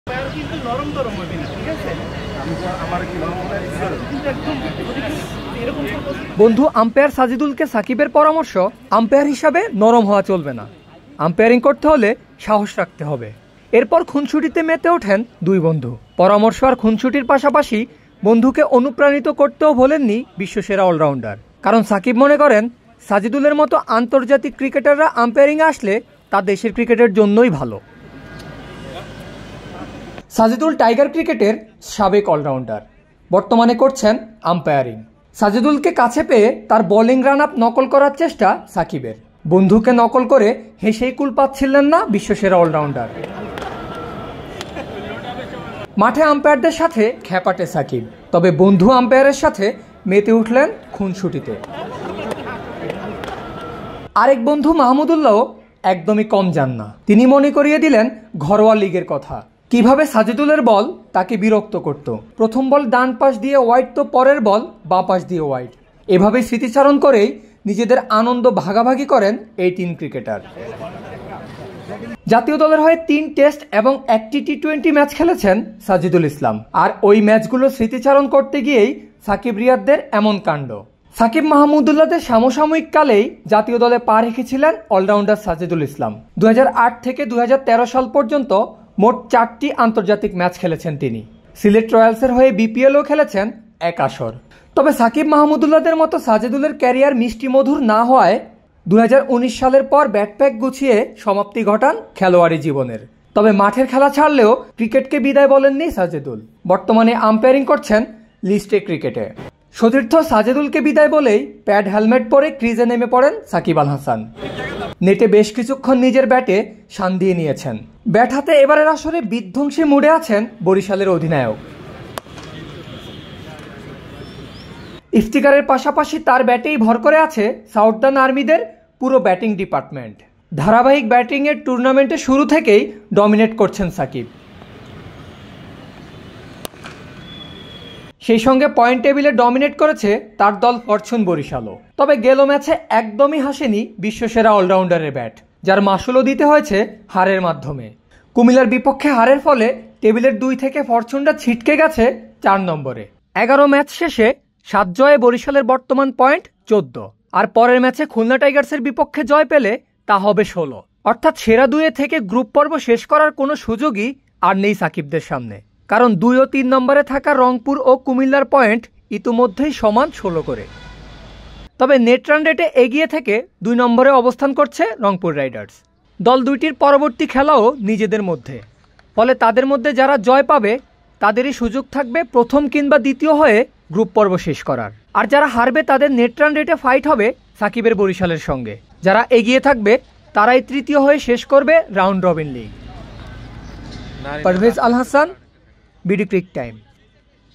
'আম্পায়ার কিন্তু নরম হবি না ঠিক আছে' বন্ধু আম্পায়ারকে সাকিবের পরামর্শ
'আম্পায়ার কিন্তু নরম হবি না'- আম্পায়ার বন্ধুকে সাকিবের টিপস